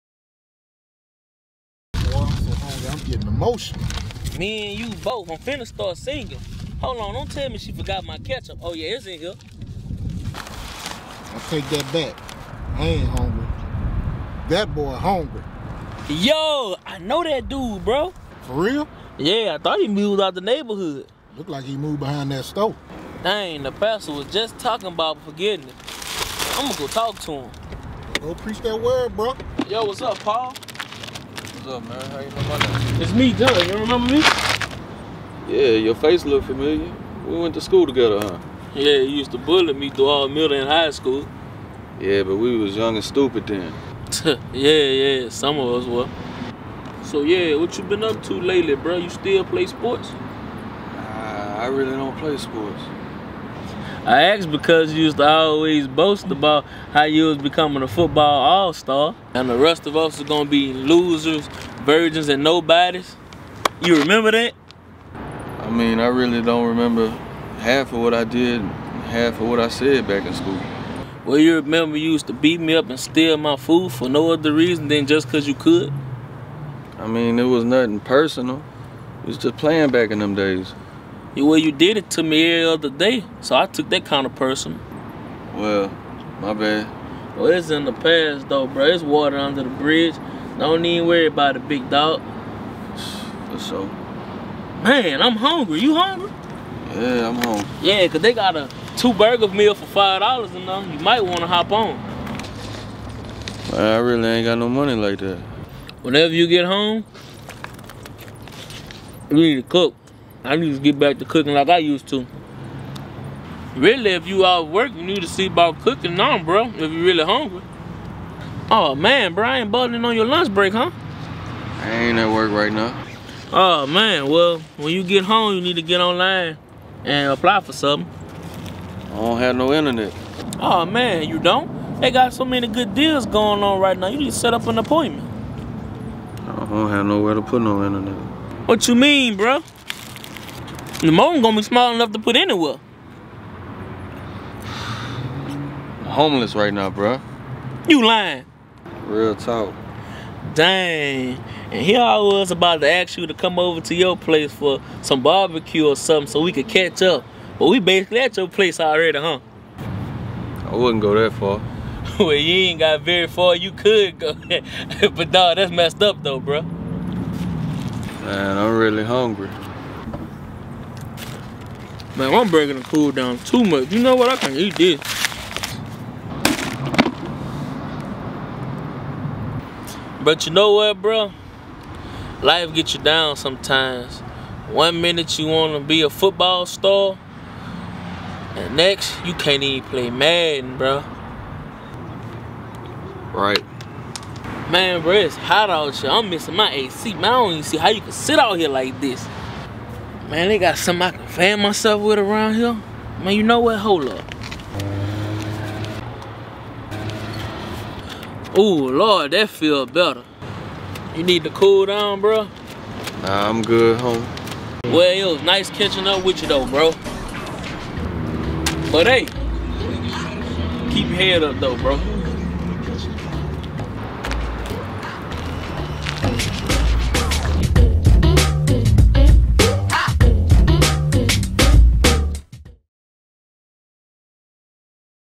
Life Gets You Down - Skit
Genre: Comedy

John catches his high school bully at his lowest point and DOESN'T choose to be the better man.